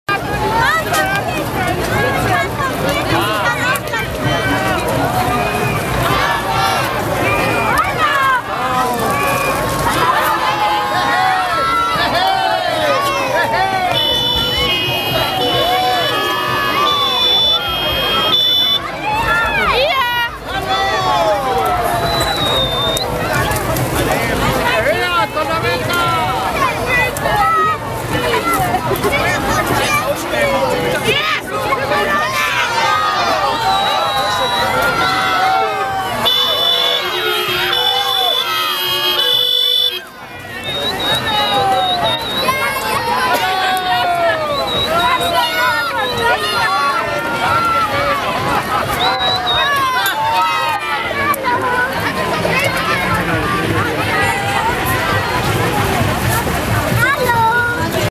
Die Blutsauger -- Rotes Kreuz Zuffenhausen, beim Fasnetumzug in Stuttgart 2012
Die Blutsauger vom Roten Kreuz Stuttgart-Zuffenhausen -- Bereitschaft und Jugend-Rotkreuz.
Beim Fasnetumzug in Stuttgart 2012